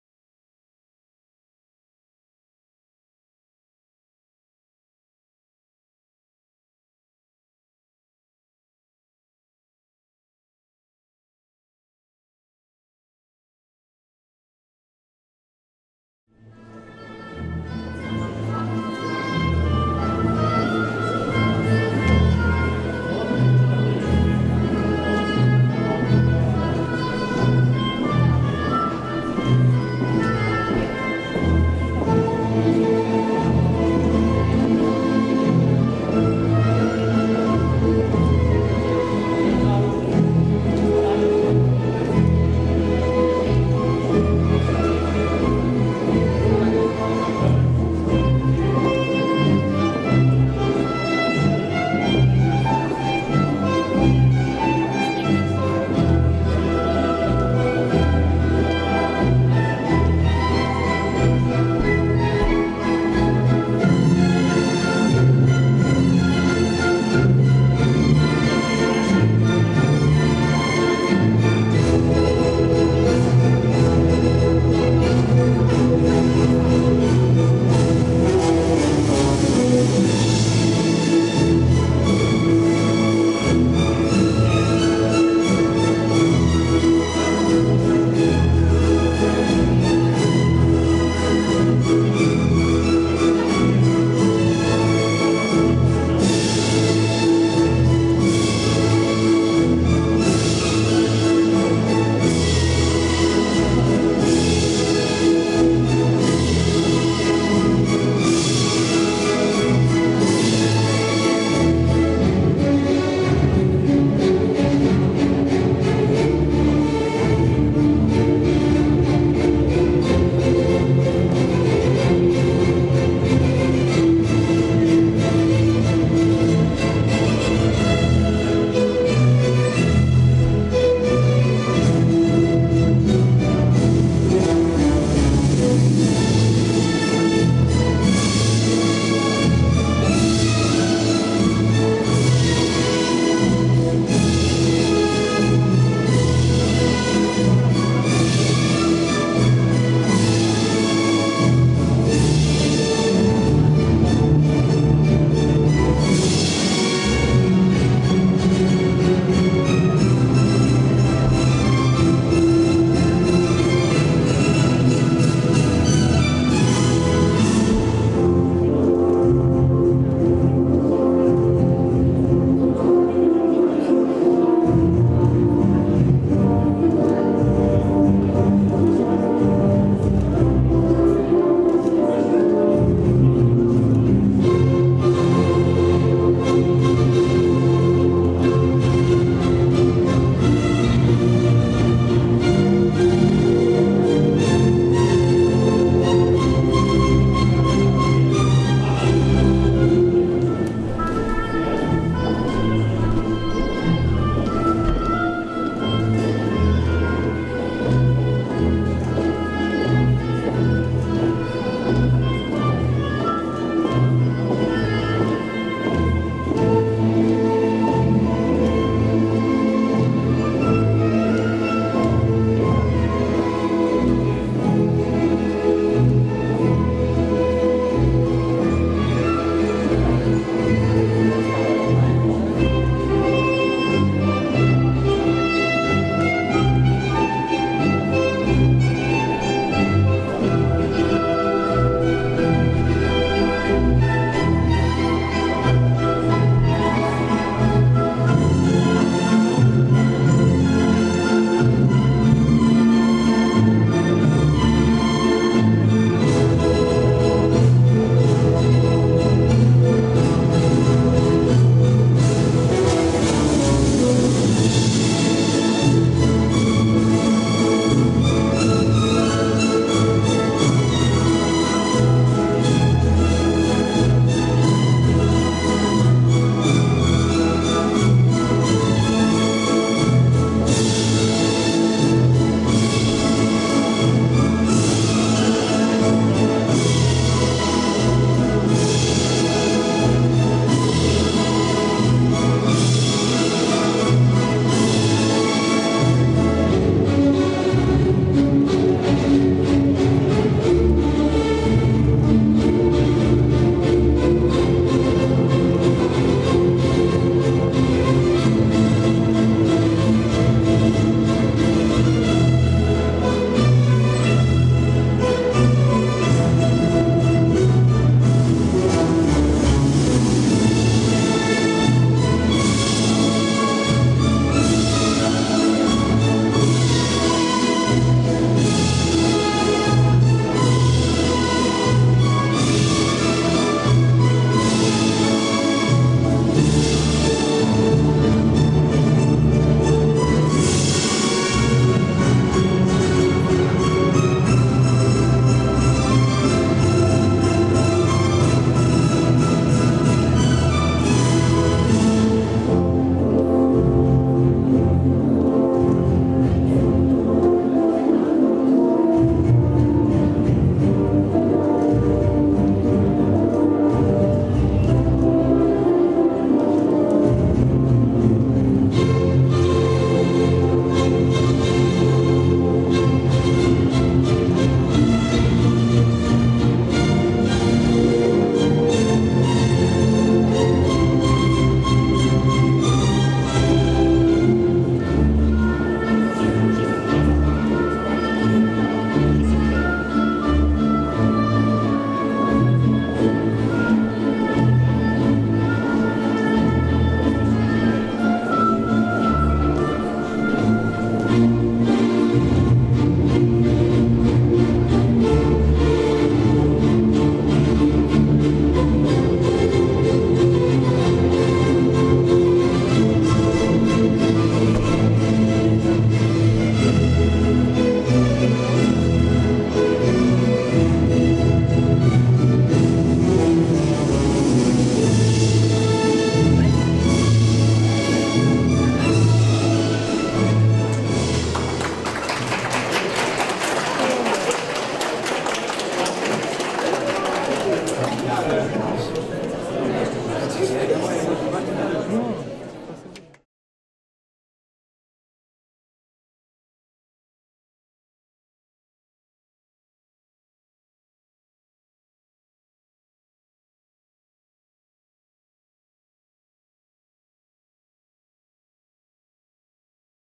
Studniówka 2019: LO im. H. Kołłątaja w Pińczowie - Polonez kl IIIC
Zapraszamy do obejrzenia fotoreportażu i relacji video ze studniówki na portalu pinczow24.pl.